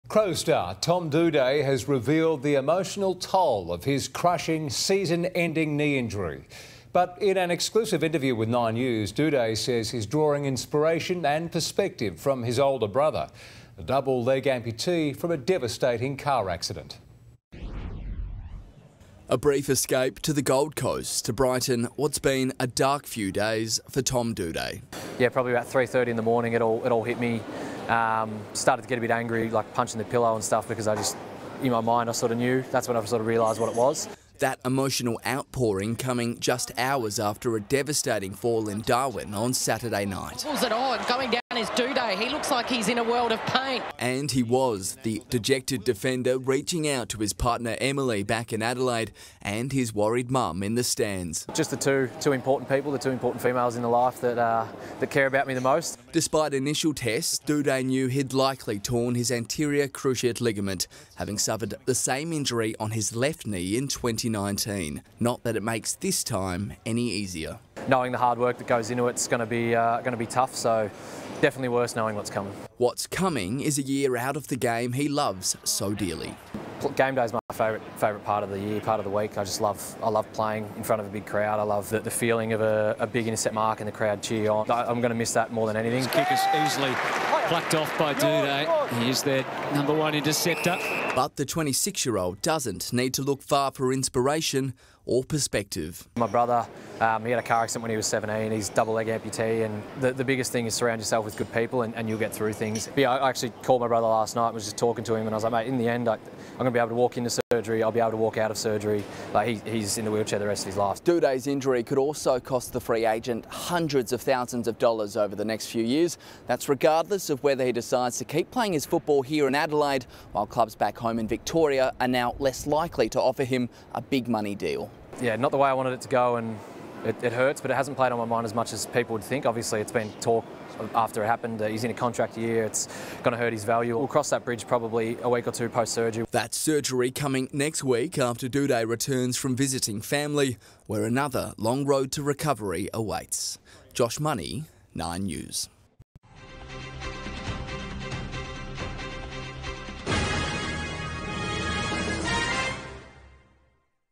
Crows star Tom Doedee reveals toll of season-ending injury | 9 News Australia
Crows star Tom Doedee has revealed the emotional toll of his crushing season-ending knee injury.